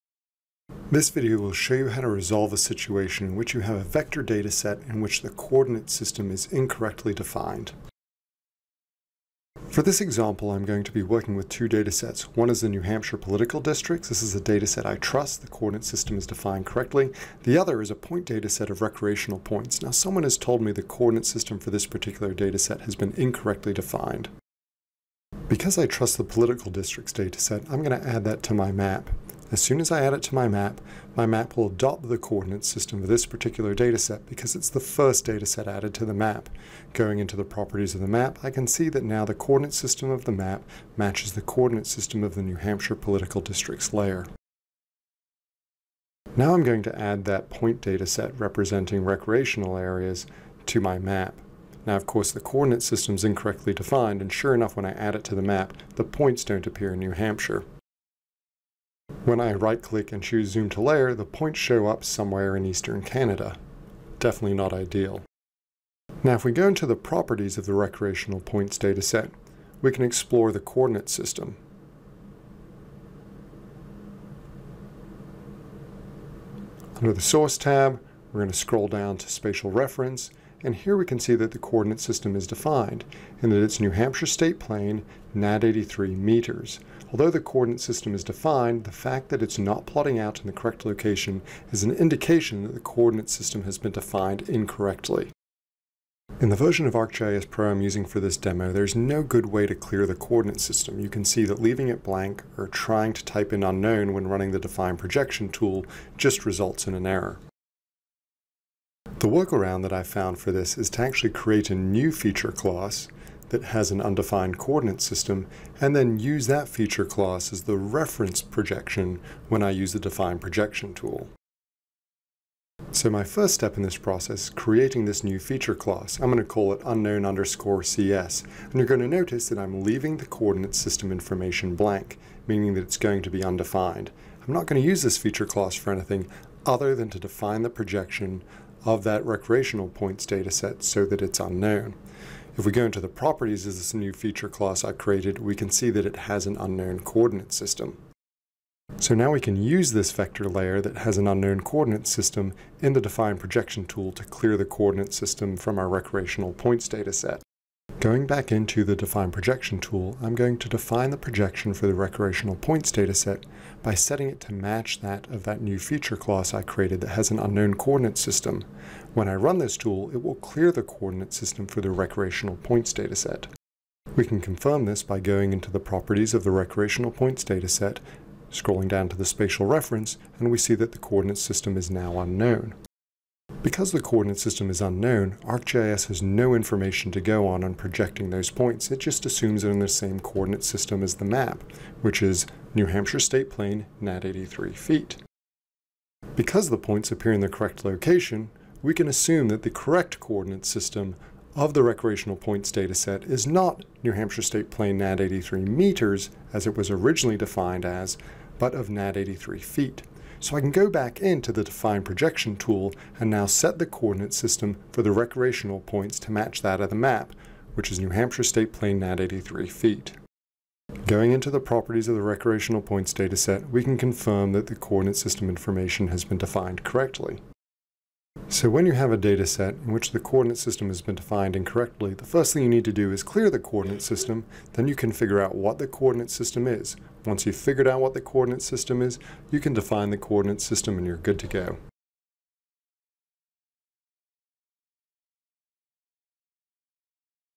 Dealing with an Incorrectly Defined Coordinate System in ArcGIS Pro
Is the coordinate system for your dataset defined incorrectly? This tutorial will show you how to go about determining the correct coordinate system in ArcGIS Pro.